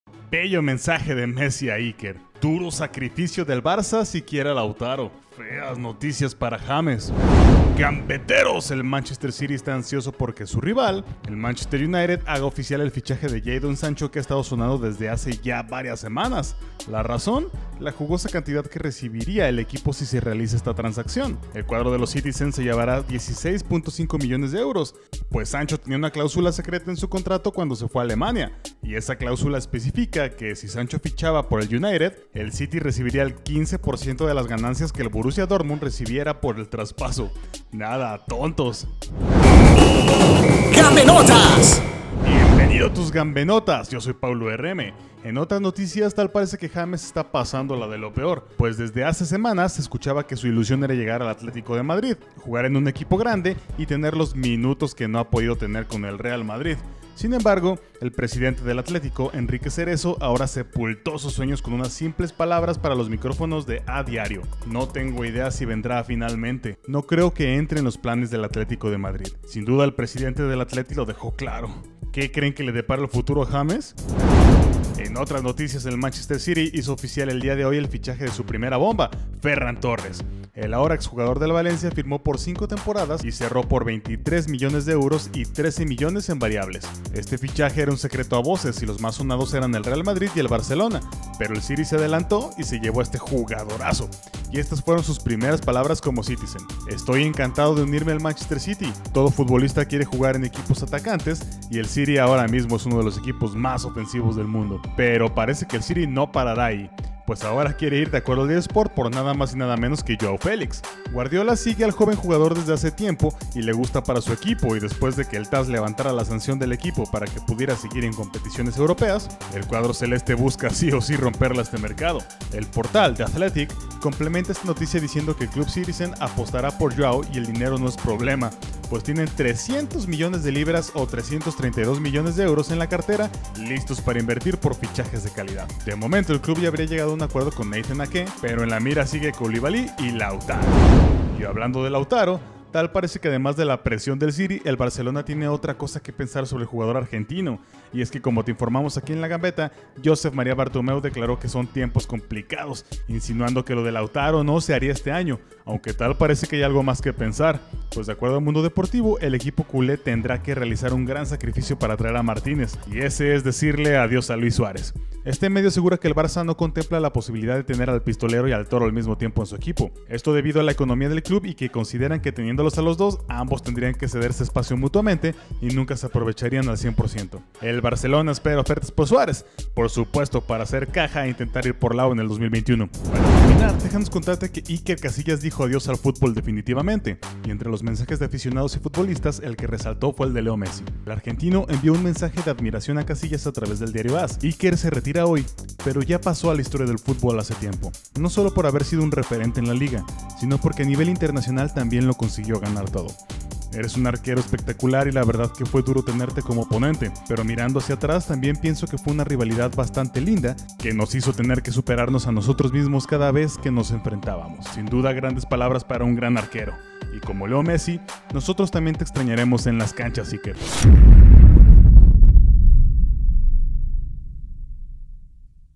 BELLO MENSAJE de Messi a Iker|DURO SACRIFICIO del Barça SI QUIERE a Lautaro|FEAS NOTICIAS para James
Hola Gambeteros, aquí les tenemos la mejor información del día en el mundo del fútbol, hoy les informamos sobre el bello mensaje que Messi le ha dedicado a Iker Casillas, además de las tremendas noticias del Atlético de Madrid respecto a james